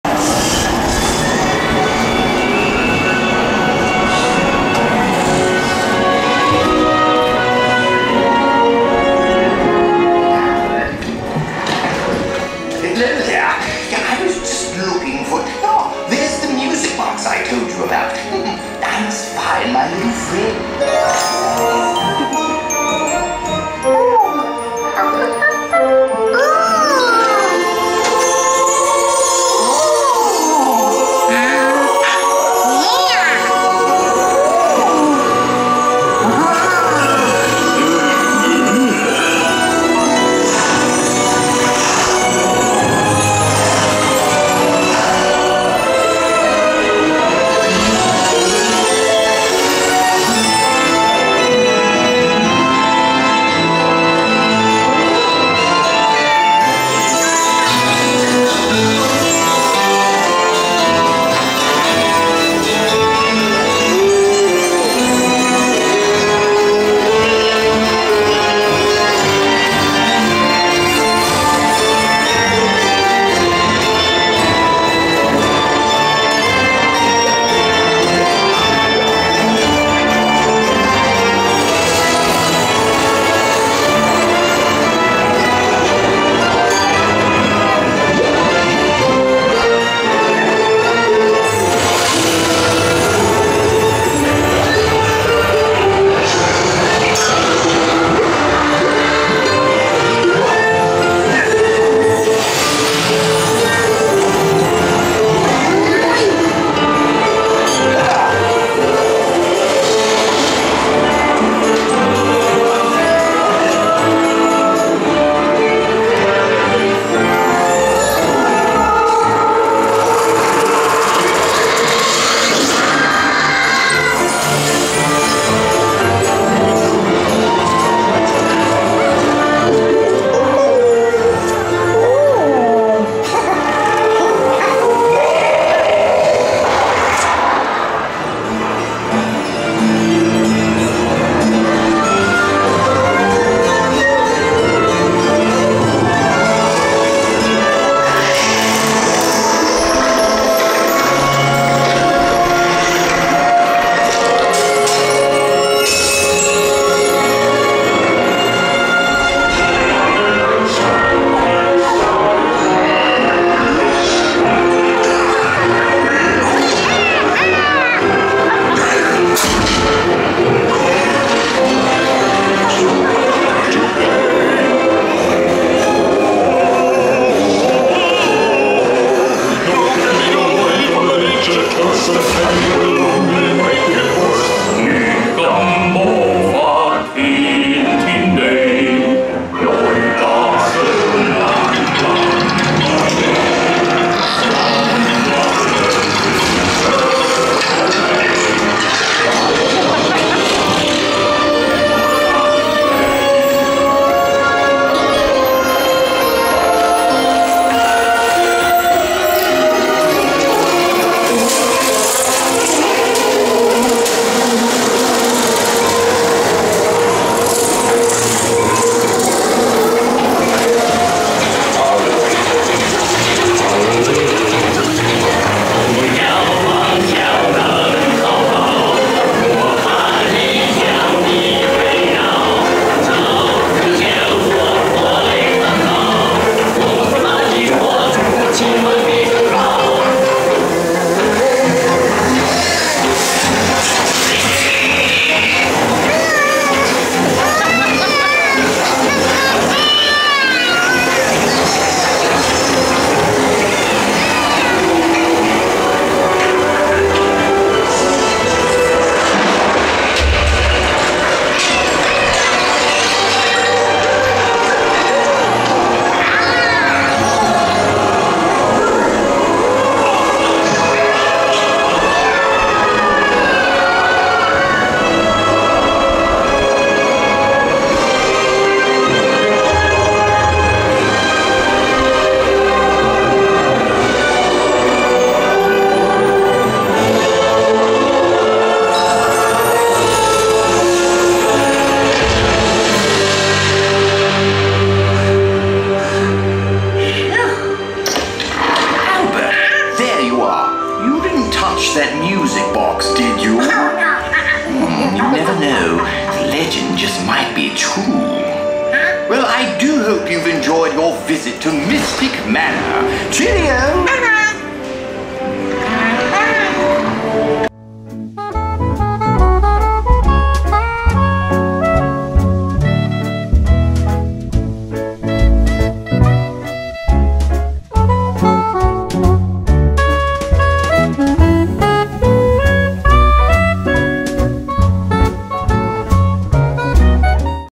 Mystic Manor POV Full Ride - Trackless Ride - Hong Kong Disneyland
In diesem Video zeige ich euch Mystic Manor !💕disneyparks

Meine Amazon Shopping Liste*:

(bezahlter Link)

Auch zukünftig möchte ich euch in meine Welt von Disney mitnehmen und euch zeigen wie es sich in mein Leben integriert!

(Dieses Video enthält unbezahlte Werbung durch Markenerkennung oder Personenmarkierungen)